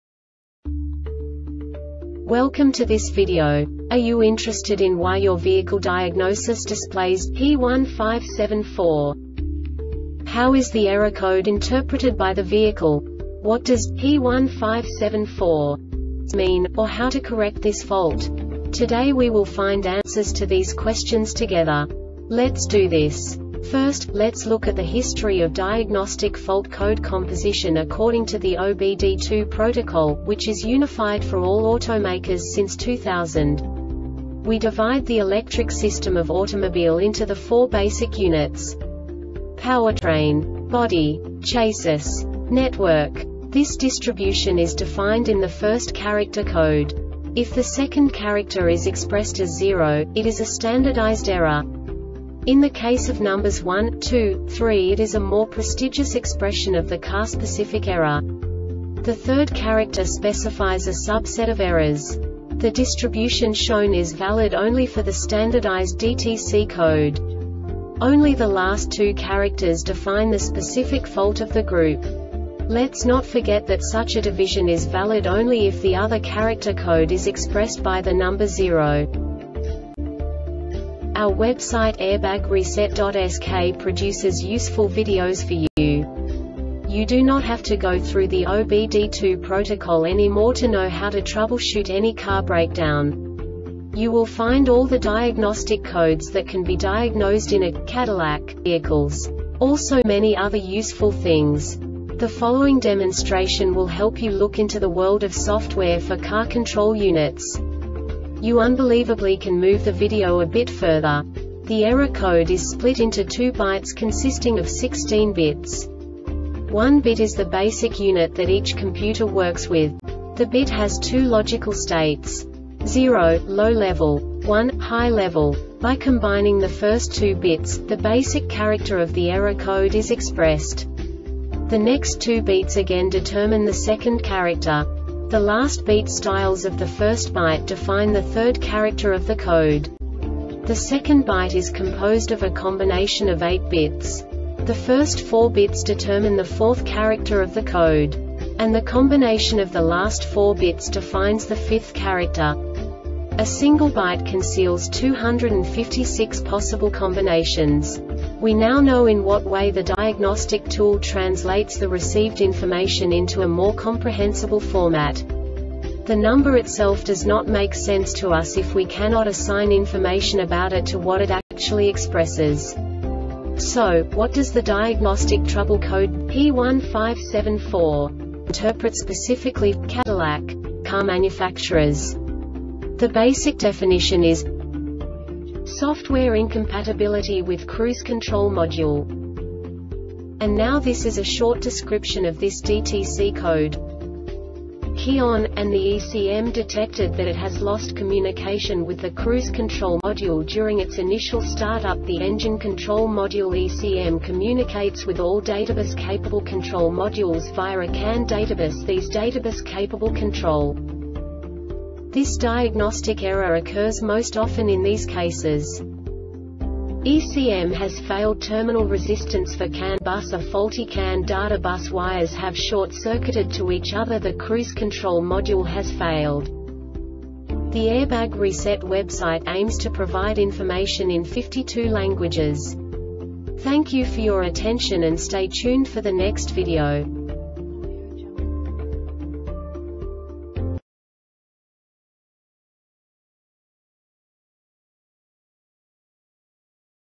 DTC cadillac P1574 Short Explanation
Contents:
0:21 Basic DTC analysis according to OBD2 protocol standard.
1:48 Insight into programming
2:58 A diagnostically understandable description of the Diagnostic Trouble Code
3:05 Basic error definition

Make:
Cadillac
Code:
P1574
Definition:
Software Incompatibility with Cruise Control Module
Description:
Key on,and the ECM detected that it has lost communication with the Cruise Control Module during its initial startup.The Engine Control Module (ECM) communicates with all databus-capable control modules via a CAN databus.These databus-capable control m
Cause:
ECM has failedTerminal resistance for CAN-bus are faultyCan data bus wires have short circuited to each otherThe cruise control module has failed